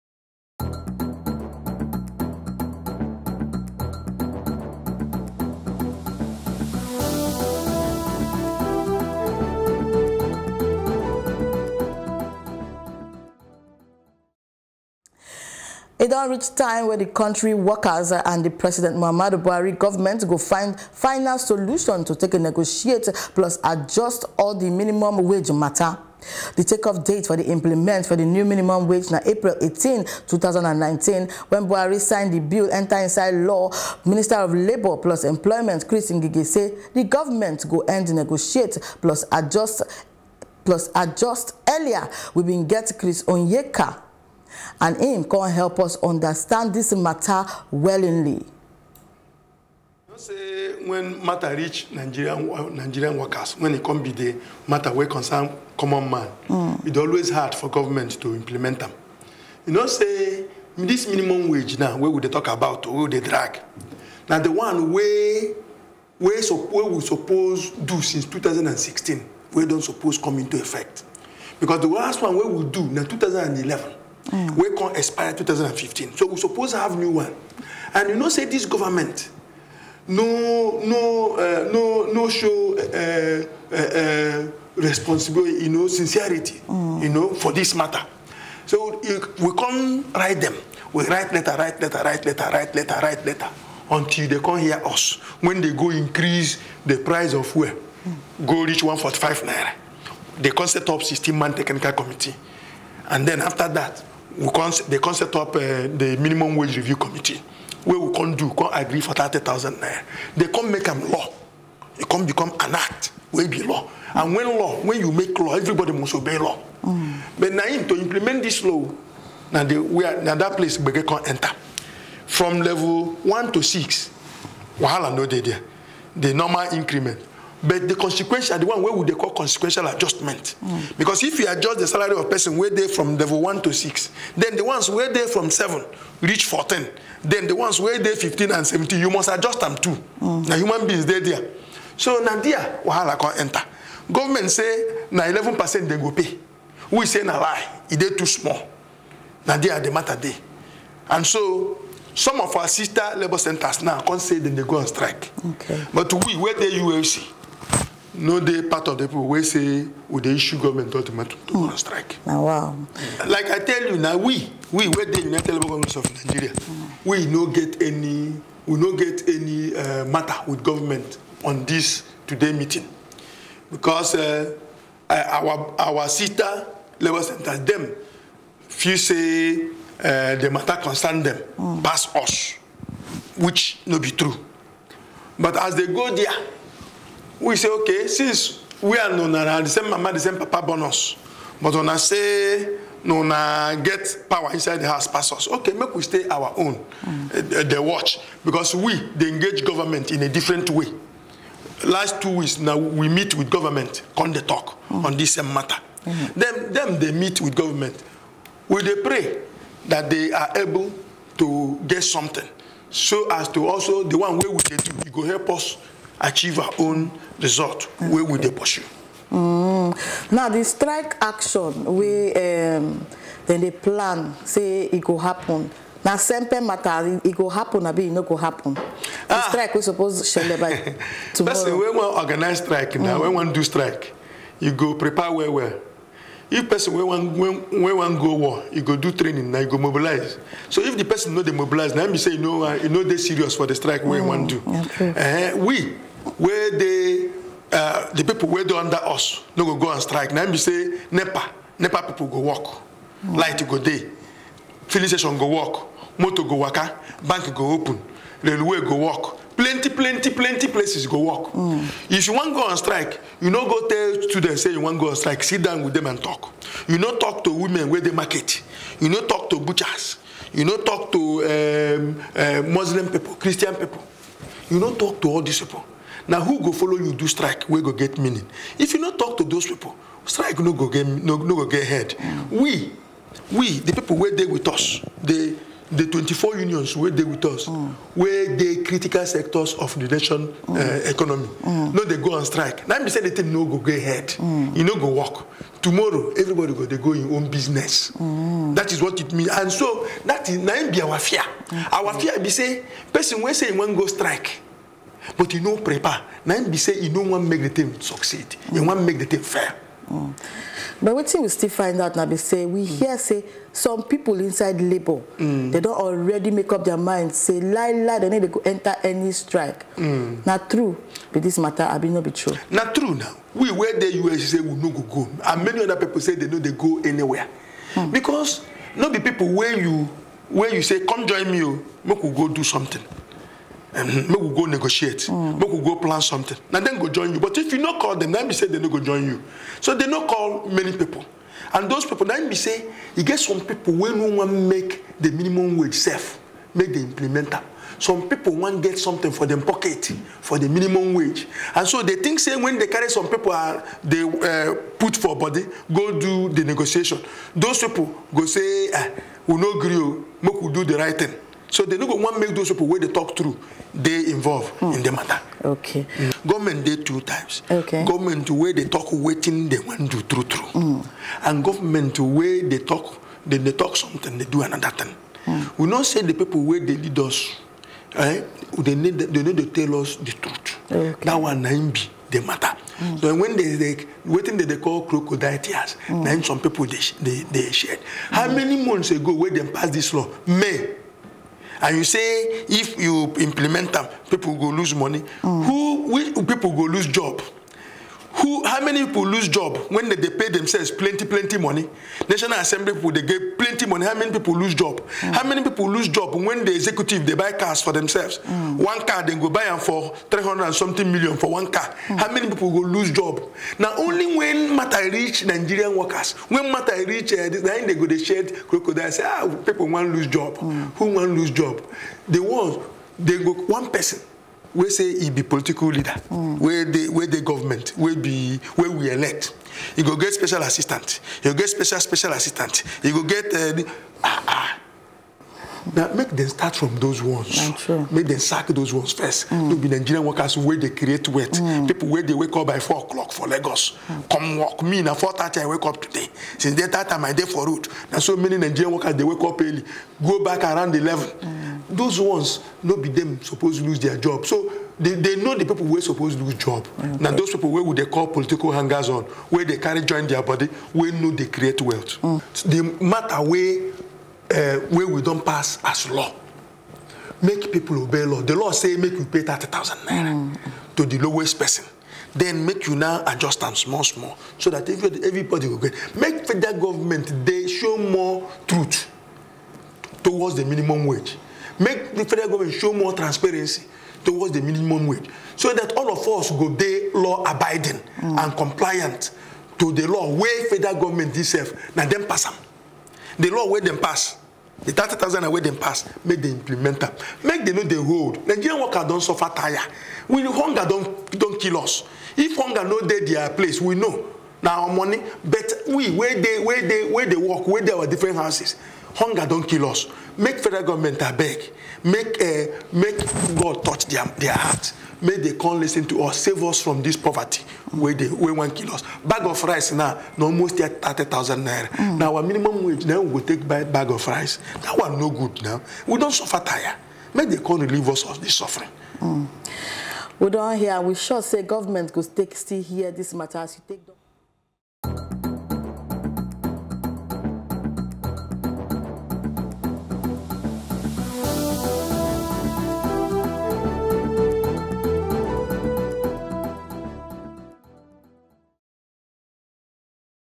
Naija Workers Don Ready For Strike As As Fed. Govt. Resume Negotiation Today
E don reach time wey the country workers and the President Muhammadu Buhari government go find solution to negotiate and adjustment of the minimum wage matter.

The take-off date for the implement of the new minimum wage na April 18, 2019 when Buhari signed the bill to law
Minister of Labour and Employment, Chris Ngige, say the government go and negotiate and adjust.